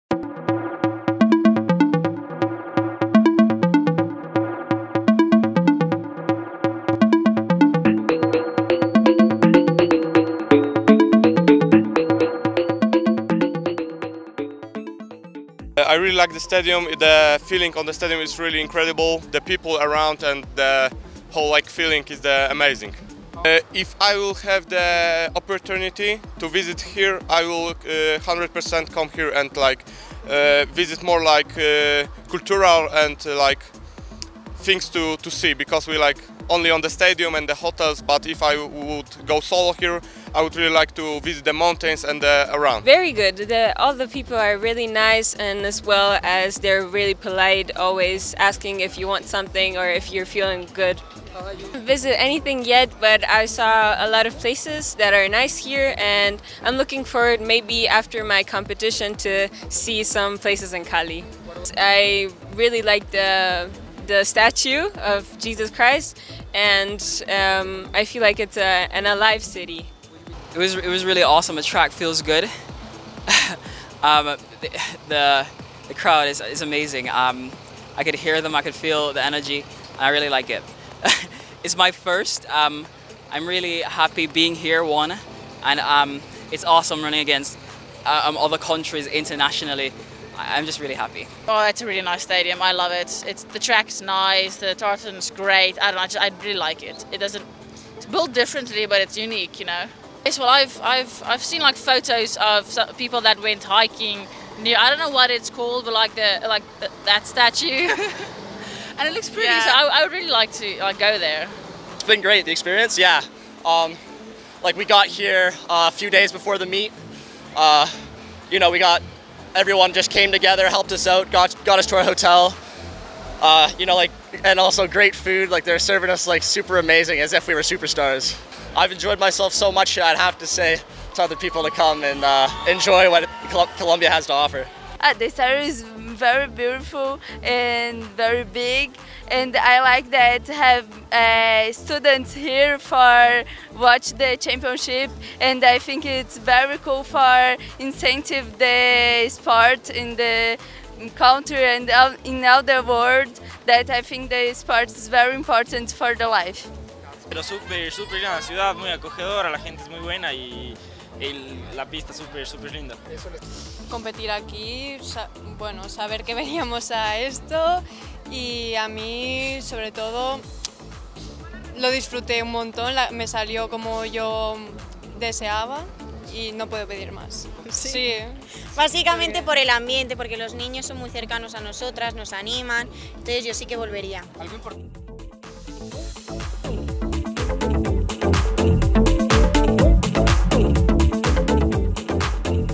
Atletas del Mundial de Atletismo sub-20 están enamorados de Cali
Las estrellas futuras del atletismo hablan de lo que ha significado para sus vidas venir a Cali, una ciudad que los recibió con los brazos abiertos.